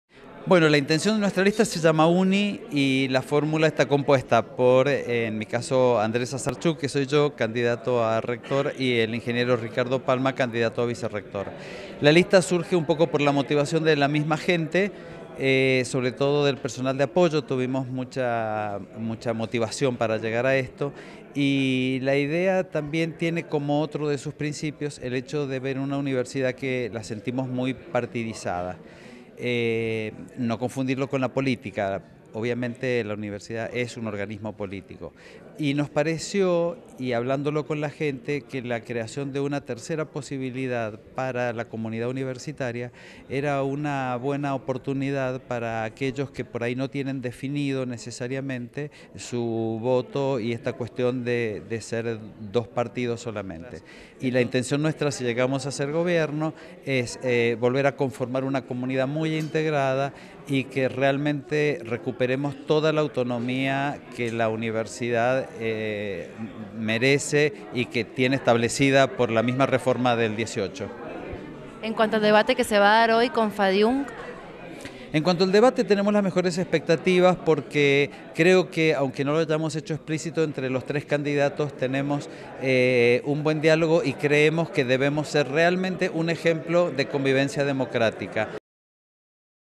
(NO USAR) Debate electoral en la UNCUYO: Andrés Asarchuk
El docente y candidato a rector de la UNCUYO, luego del debate oficial entre los candidatos, que se ofreció en el Comedor Universitario, hizo un balance de su campaña y contó cómo se prepara para el jueves 7, día del sufragio.